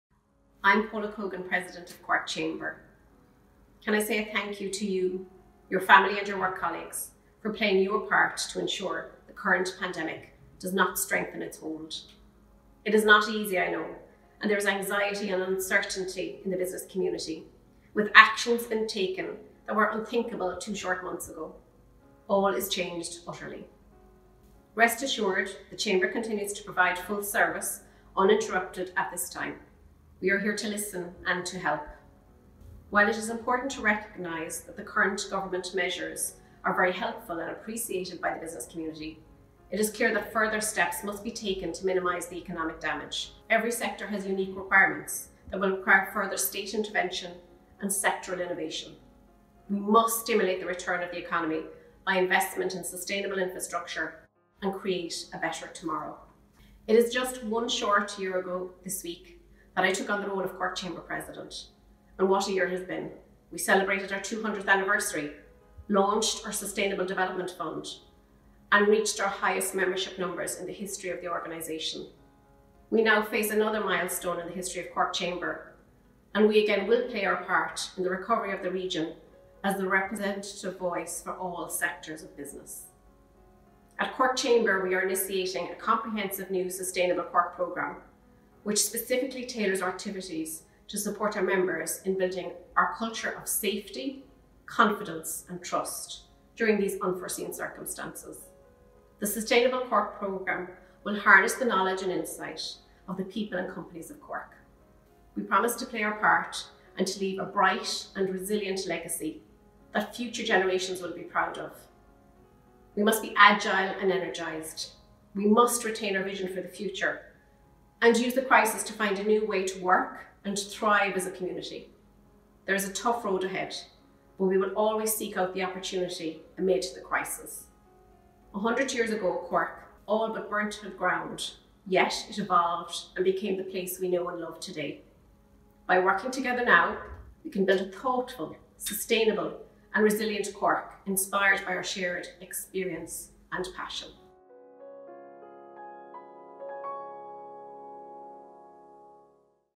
Building Resilience
Paula Cogan, President of Cork Chamber addresses the business community of Cork as the region begins to plan to reopen the economy.